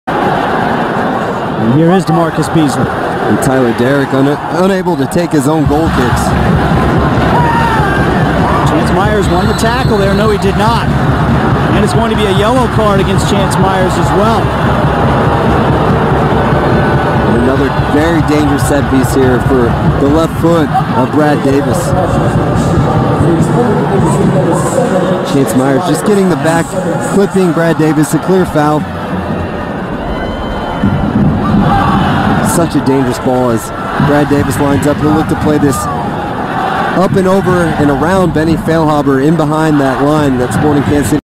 Tackling foul/UB reckless foul 75m
Sporting Kansas City vs Houston Dynamo
1 August 2015
Referee: Jorge Gonzalez
Players: Chance Myers (Sporting KC), Brad Davis (Houston Dynamo)